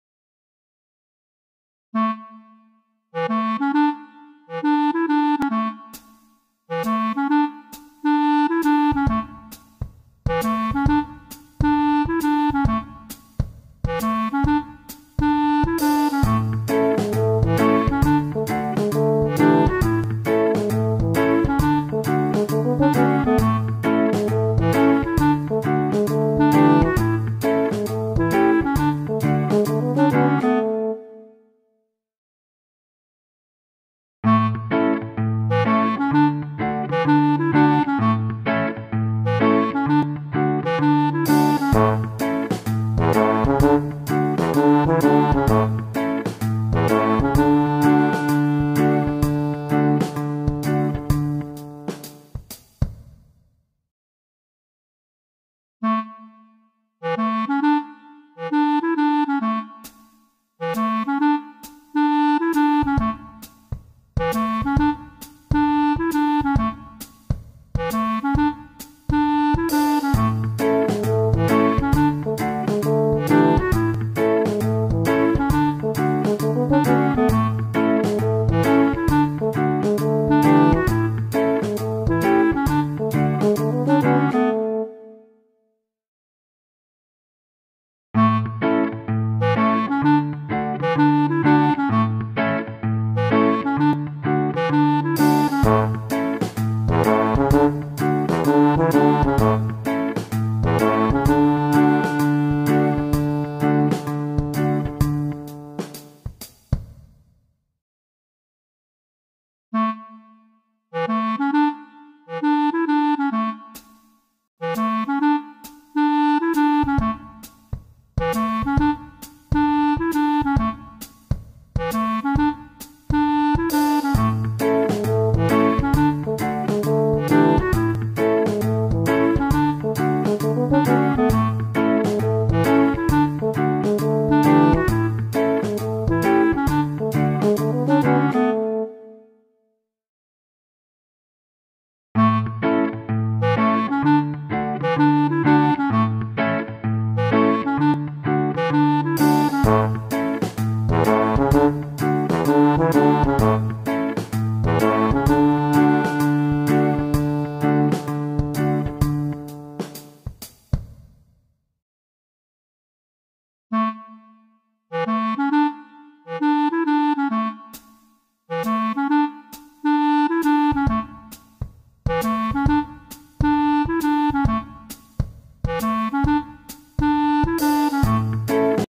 Sample Video Intro for Online E-Classroom and Interactive SIM in Mathematics ..(contents to follow)
Sample Video Intro for an Online E-Classroom and Interactive SIM in Mathematics - Flexible Learning Management Systems of the "New Normal". 😇 (contents to follow)